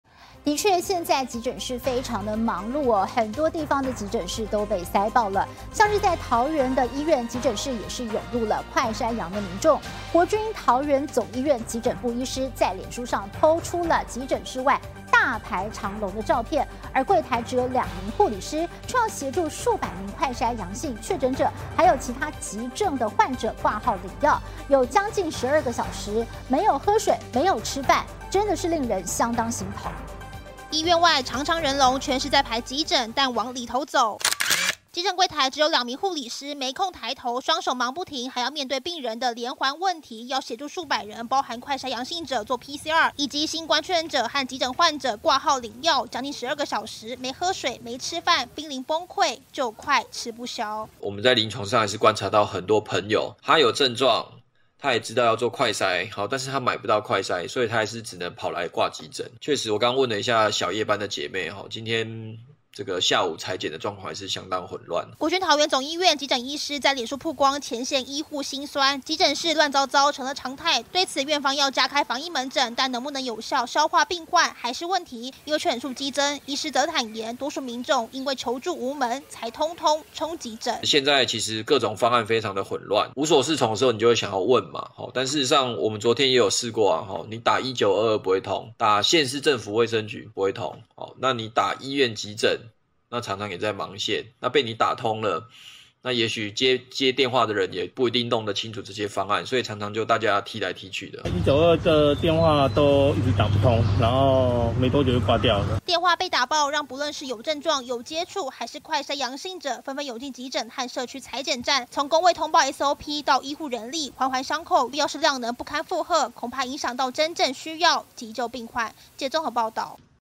白衣天使也哭泣! 忙到12hr沒喝水.沒吃飯
國內本土病例連日破萬，不僅雙北急診被塞爆，桃園的醫院急診也湧入「快篩陽」民眾。國軍桃園總醫院急診部醫師、在臉書PO出急診室外大排長龍的照片，而櫃台只有2名護理師，卻要協助數百名快篩陽性、確診者，和其他急症患者掛號領藥，將近12個小時沒喝水、沒吃飯，令人相當心疼。